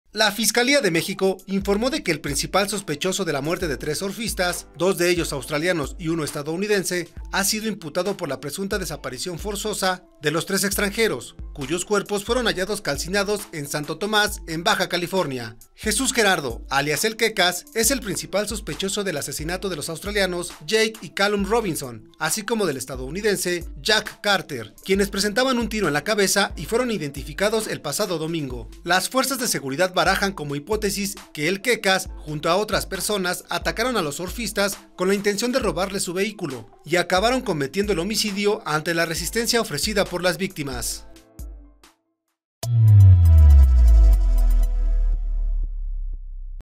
Imputan al principal sospechoso de la muerte de tres surfistas en Ensenada
La Fiscalía de México informó este jueves de que el principal sospechoso de la muerte de tres surfistas, dos de ellos australianos y uno estadounidense, ha sido imputado por la presunta desaparición forzosa de los tres extranjeros, cuyos cuerpos fueron hallados calcinados en Santo Tomás, en Baja California.